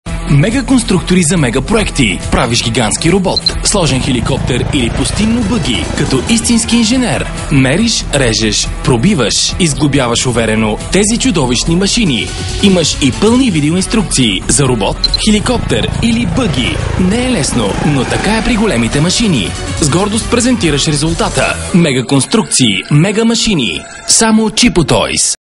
Mega Machines
Мега констуктори за мега проекти.
Правиш гигантски робот,
Сложен хеликоптер,
Или Пустинно бъги.
Като истински инженер,
Мериш, режеш, пробиваш
И сглобяваш уверено
Тези чудовищни машини.
Имаш и пълни видео инструкции,
За робот,
Хеликоптер
или бъги.
не е лесно,
но така е при големите машини.
с гордост презентираш резултата
Мега конструкции, Мега машини,
Само от чипо тойс.